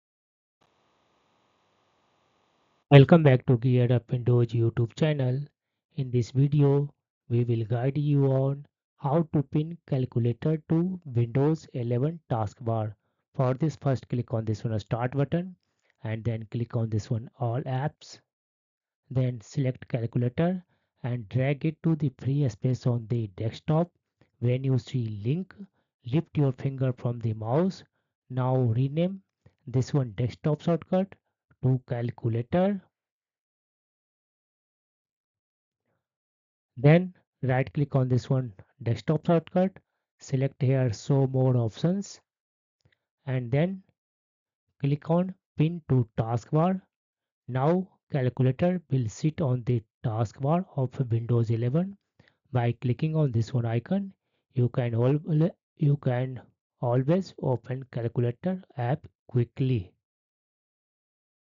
How to Pin Calculator to Windows 11 Taskbar | GearUpWindows Tutorial
Title: "Easy Guide: Pinning Calculator to Windows 11 Taskbar | GearUpWindows Tutorial"

Description:
Welcome back to GearUpWindows! In this step-by-step tutorial, we'll show you how to pin the Calculator app to your Windows 11 taskbar for quick access and convenience.

Windows 11 brings a fresh interface and enhanced functionalities, including the revamped Calculator app. Pinning it to your taskbar ensures it's always at your fingertips for instant calculations without the need to search through the Start menu.

Follow along as we demonstrate the simple process to pin Calculator to your Windows 11 taskbar. Whether you're a student, professional, or just someone who frequently needs to crunch numbers, this tip will streamline your workflow and make accessing the Calculator app a breeze.

Let's get started on optimizing your Windows 11 experience with GearUpWindows!